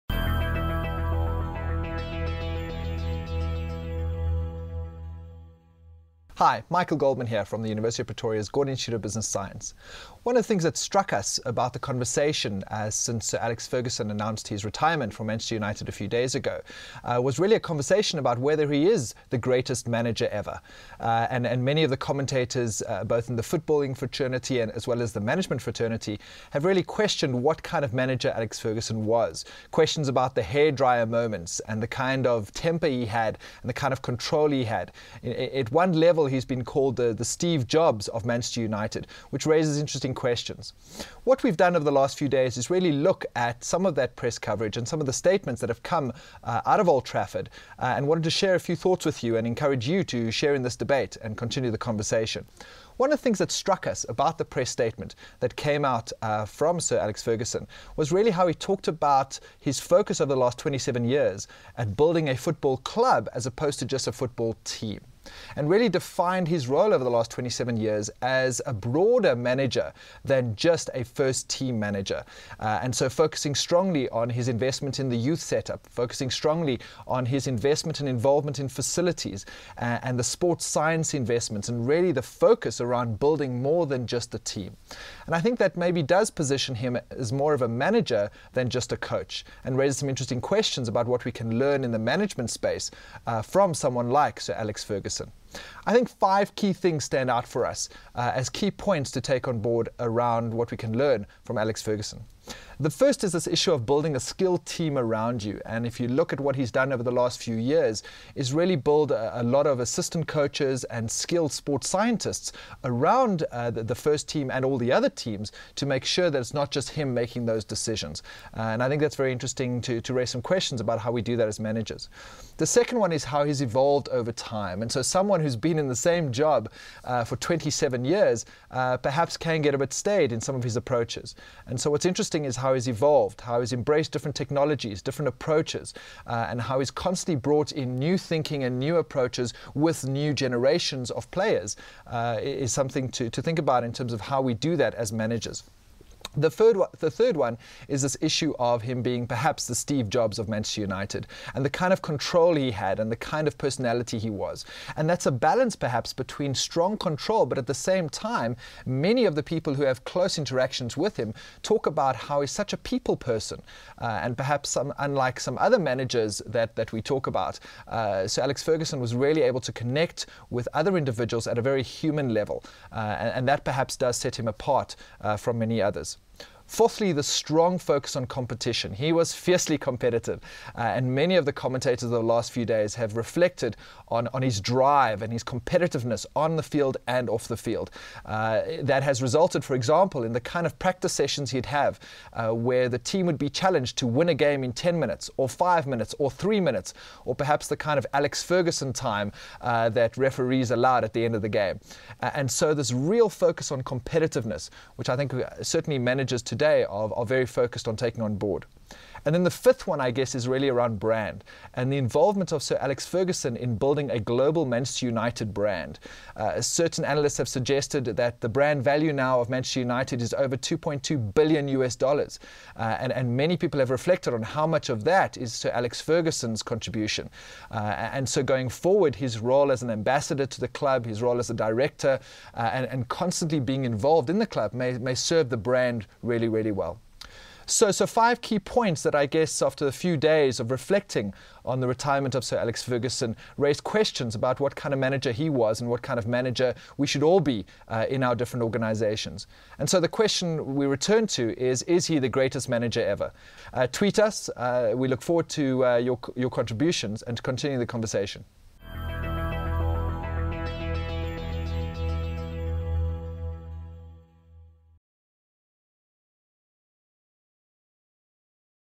Sir Alex Ferguson's Legacy
Michael Goldman, GIBS Senior lecturer, asks whether Sir Alex Ferguson is the greatest manager ever.